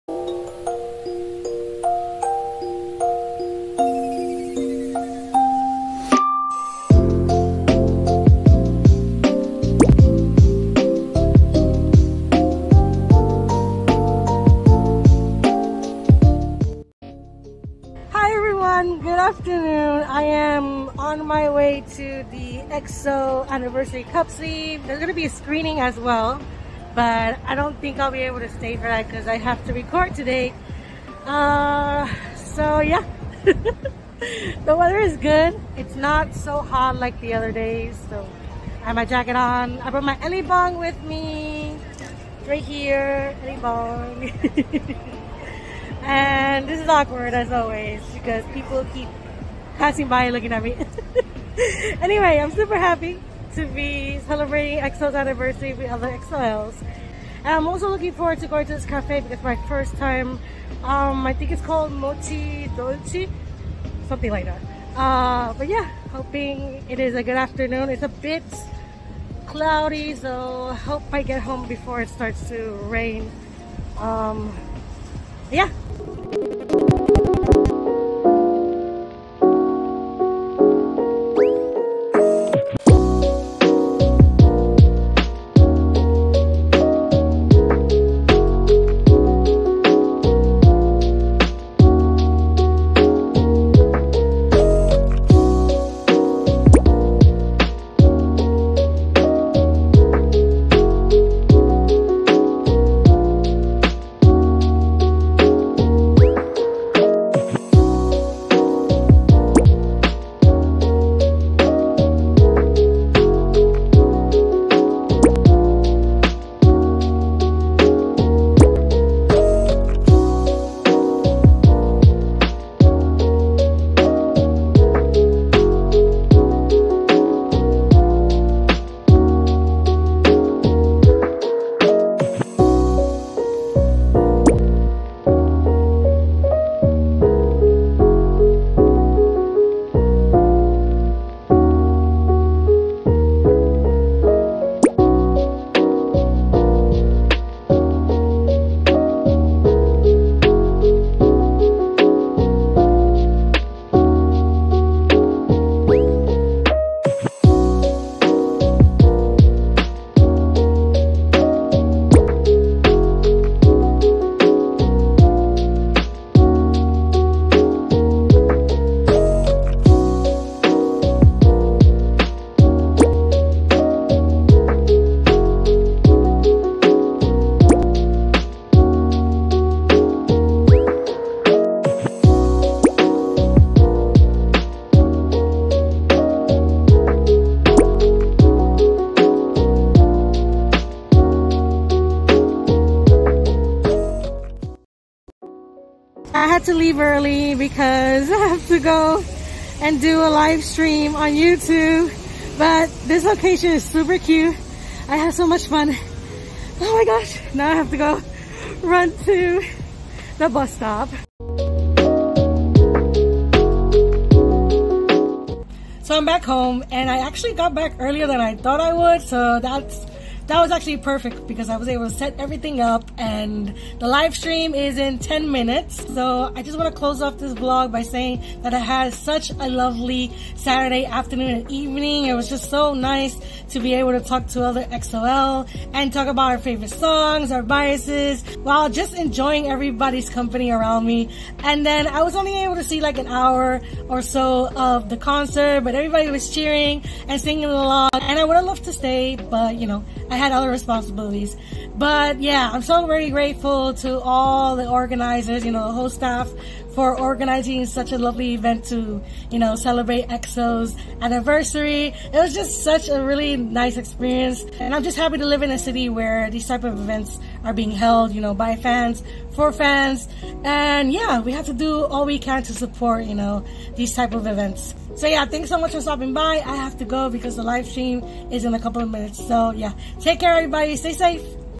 exo-l mini vlog 🦄 | EXO 11TH ANNIVERSARY CUPSLEEVE EVENT + WATCH PARTY in NYC✨
So happy to share with you all my mini vlog at the EXO 11th anniversary cupsleeve event and watch party. Such a cute event! I'm glad to have spent some time celebrating EXO with fellow EXO-L. 🤍☺️

─── ･ ｡ﾟ☆: .☽ . :☆ﾟ. ───

─── ･ ｡ﾟ☆: *.☽ .* :☆ﾟ. ───